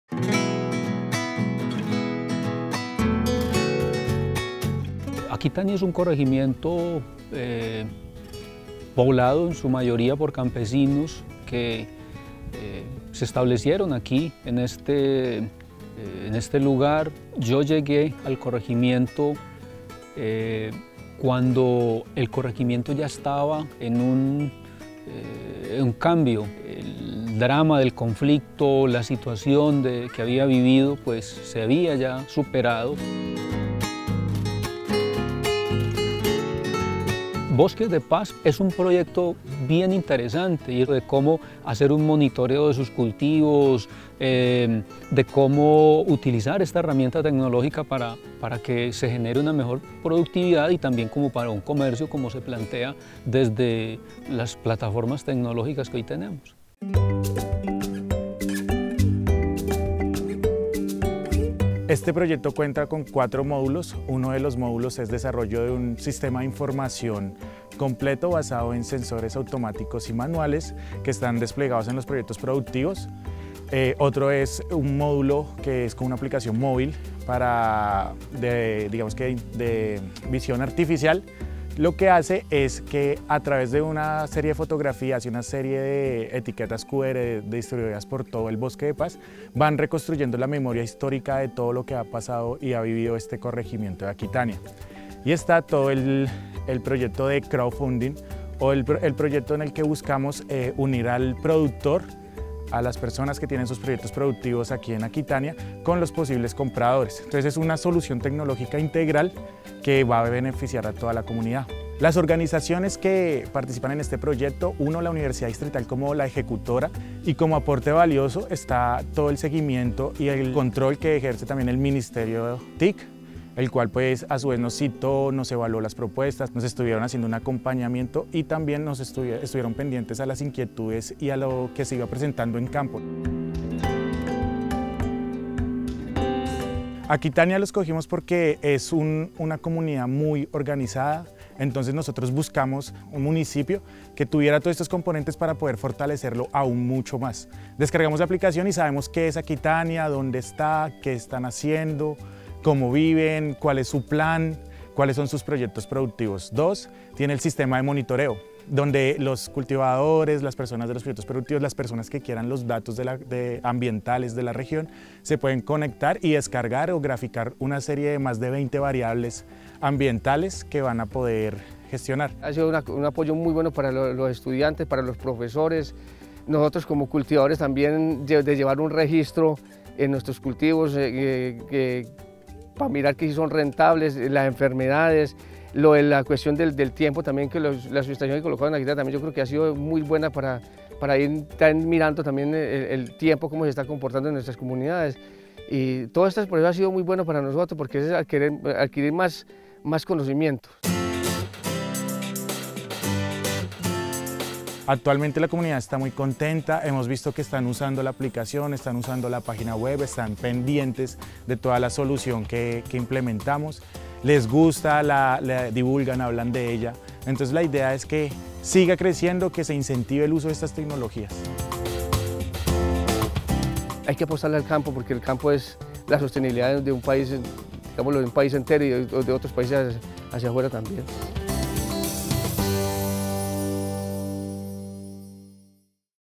En Aquitania, la tecnología facilita el comercio y la memoria histórica C45-N5
Con la estrategia TIC y Agro fortalecemos el trabajo en las regiones más vulnerables. Por eso, fuimos hasta Aquitania, Antioquia, donde estudiantes implementaron un sistema de sensores para mejorar los cultivos de los campesinos.